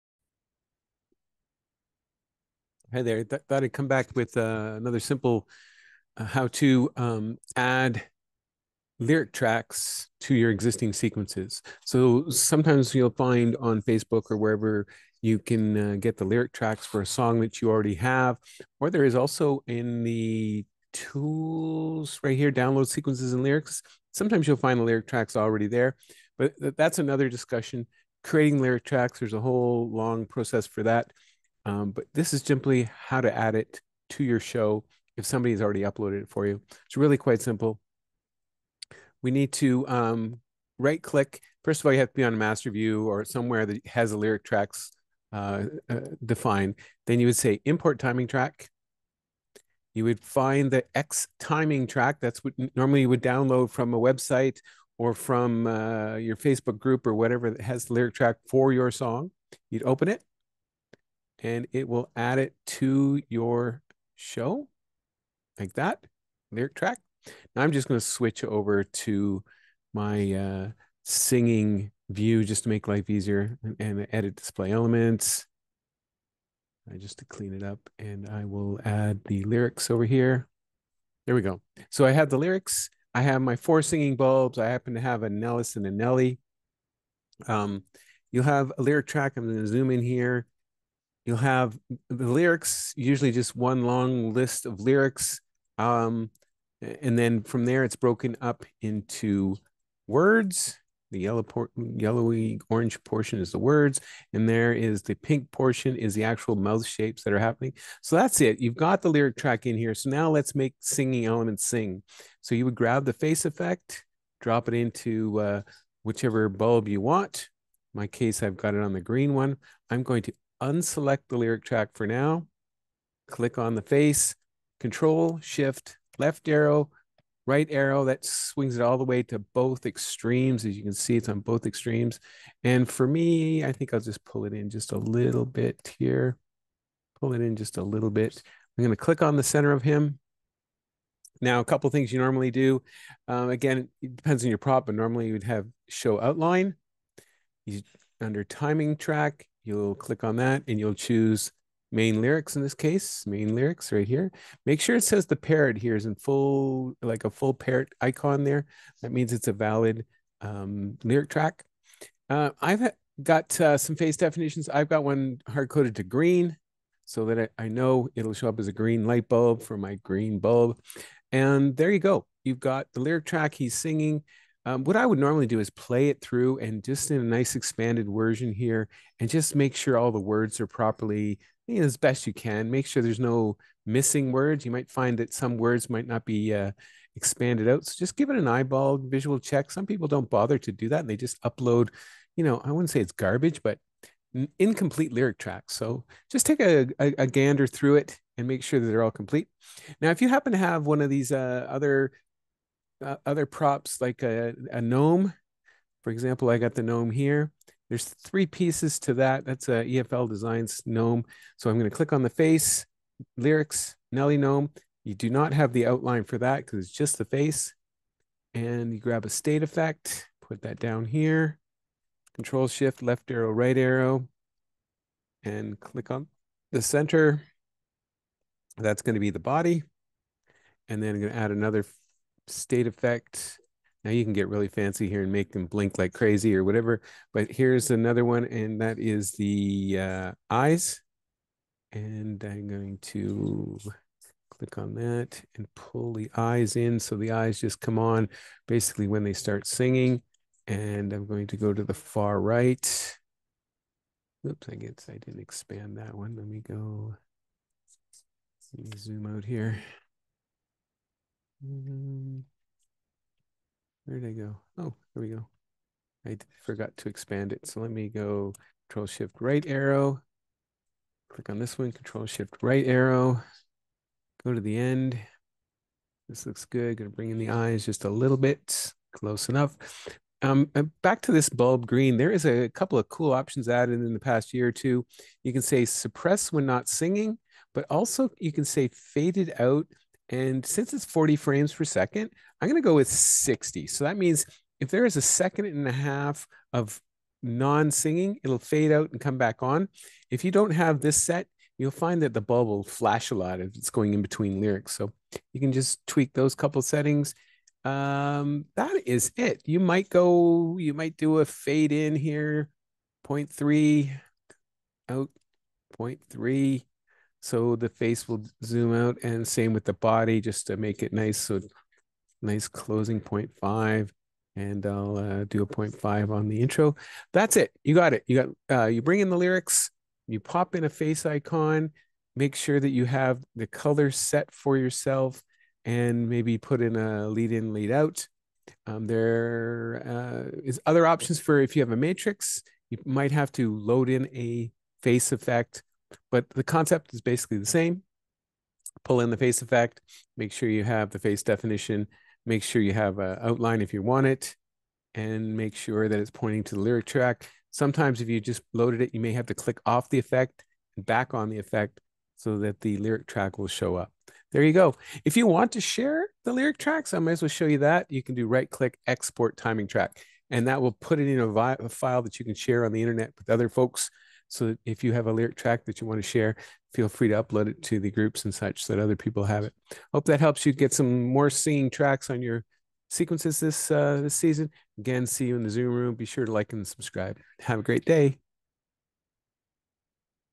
xLights Bonus Gem: Adding lyric tracks to your sequences
How to add lyric tracks to your existing sequences and connect them to your singing bulbs or singing props.